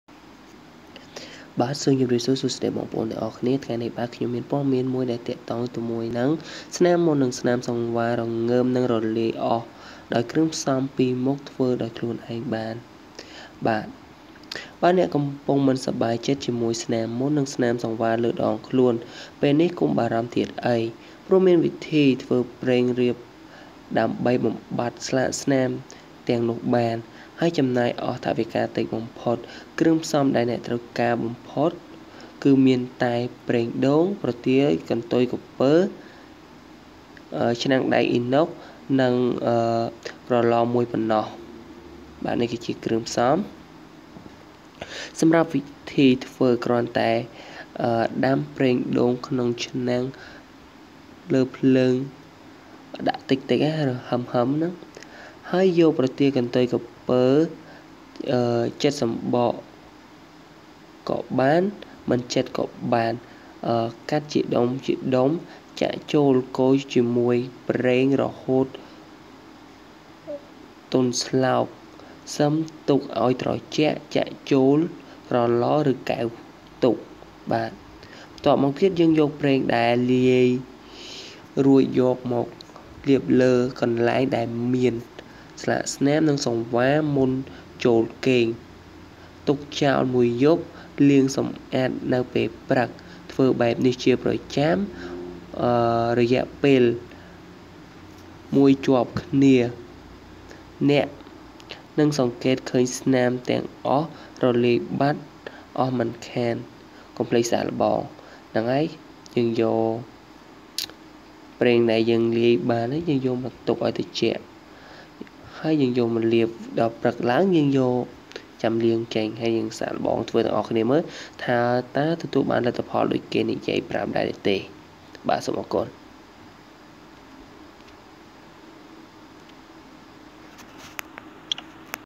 ស្នាម​មុន​និង​សង្វារ​រងើម​នឹង​រលាយ​អស់​ដោយ​គ្រឿង​ផ្សំ​២​មុខ ធ្វើ​ខ្លួន​​ឯង​បាន​
Cambodai ស្នាម​មុន​និង​សង្វារ​រងើម​នឹង​រលាយ​អស់​ដោយ​គ្រឿង​ផ្សំ​២​មុខ ធ្វើ​ខ្លួន​​ឯង​បាន​,